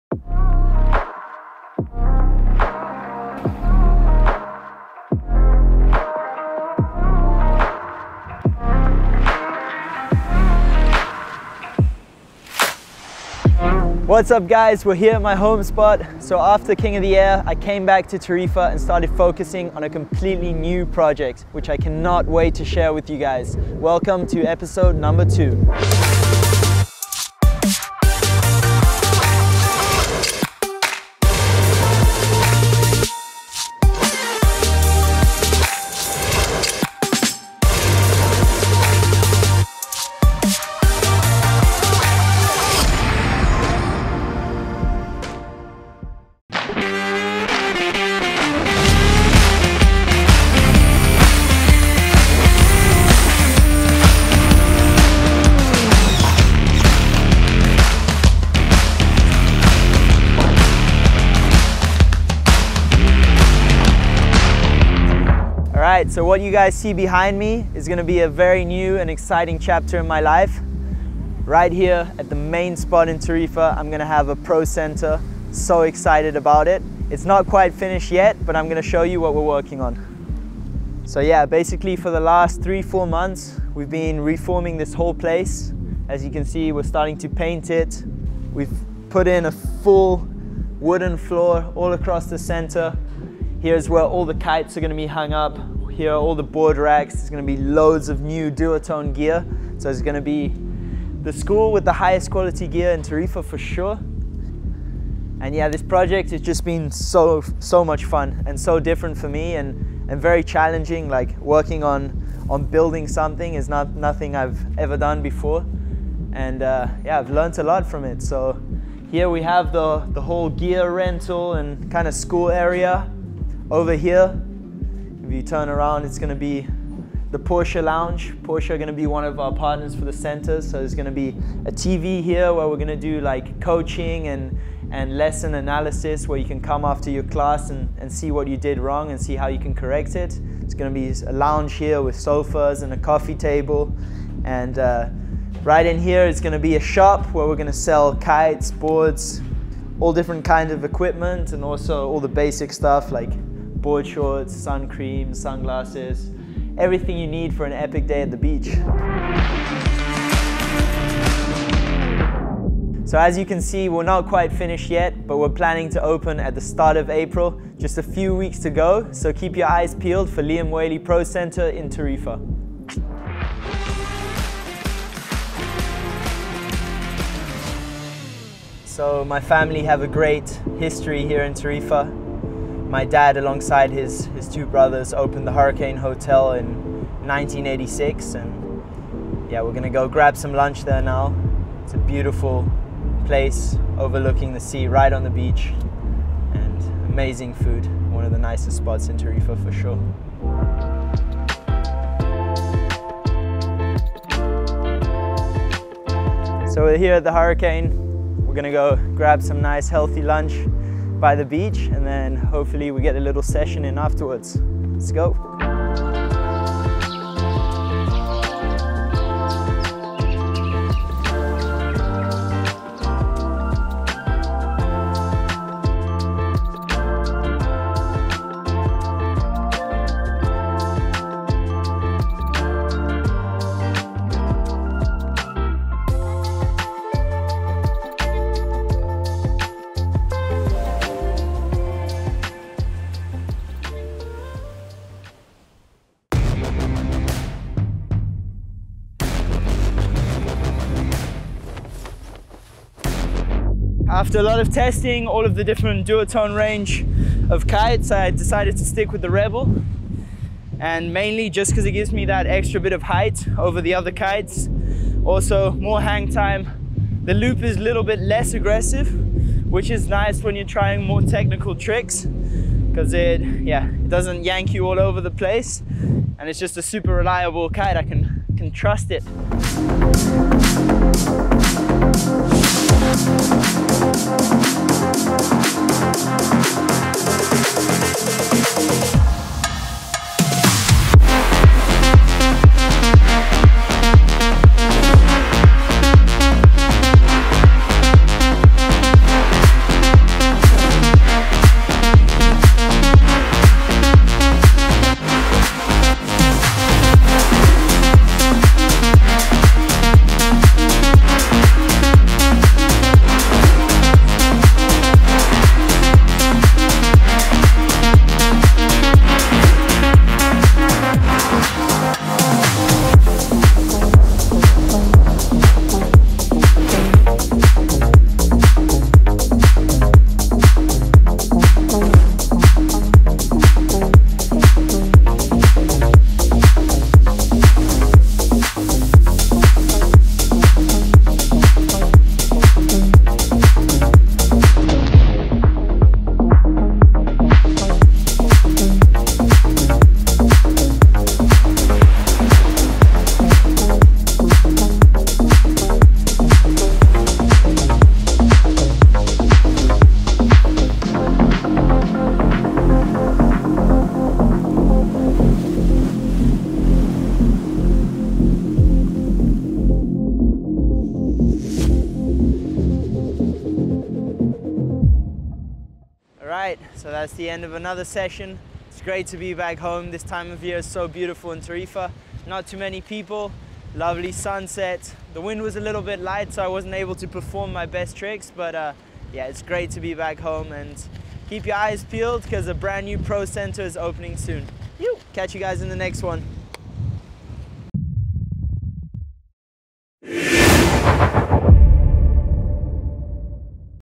LIAM WHALEY PRO CENTER, coming soon! - World Of Whaley³ - Episode 2
After the competition season came to an end It was time to come back home to Tarifa and spend the winter working on a completely new and different project. I am proud to announce that I will be opening my own Pro Center in April. We will be located at the main beach in Tarifa called Valdevaqueros which is right opposite the famous beach bar Tumbao. We will offer Kitesurfing, Windsurfing & Wing Lessons and rental as well as having a shop with all the latest gear. Alongside our partners Duotone and Porsche we hope to make this one of the most elite water sports centres in the world!

Super excited to start this next chapter in my life. More news about the project coming soon!

Filmmaker: Sebastian Porral

Location: Tarifa, Spain

Episode Partners:  @Porsche     @DUOTONEKiteboarding